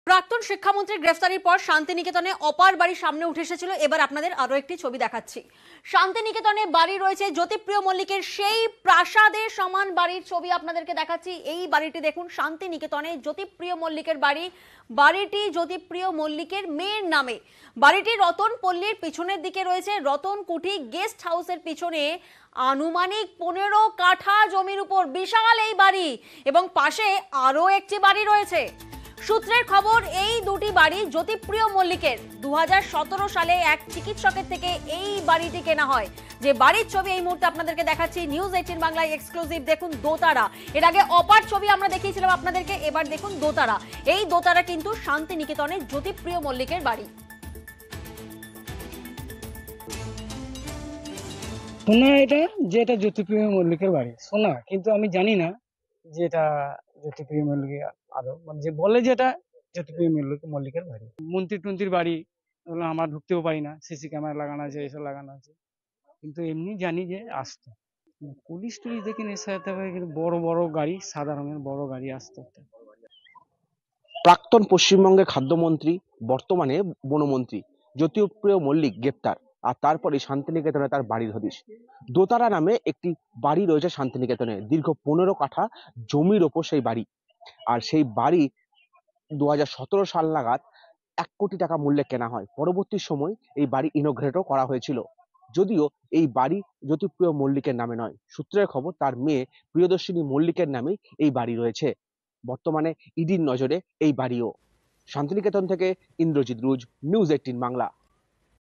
Jyotipriya Mallick: Shantiniketan এ জ্যোতিপ্রিয়রও প্রাসাদের মতো বাড়ি! দেখলে অবাক হবেন | Bangla News
Jyotipriya Mallick: Shantiniketan এ বাড়ি রয়েছে জ্যোতিপ্রিয় মল্লিকের। বাড়িটি জ্যোতিপ্রিয় মল্লিকের মেয়ের নামে। বাড়িটি রতন পল্লির পিছনের দিকে। রতন কুঠি গেস্ট হাউসের পিছনে আনুমানিক ১৫ কাঠা জমির উপর বিশাল বাড়ি এবং পাশে আরেকটি বাড়ি রয়েছে।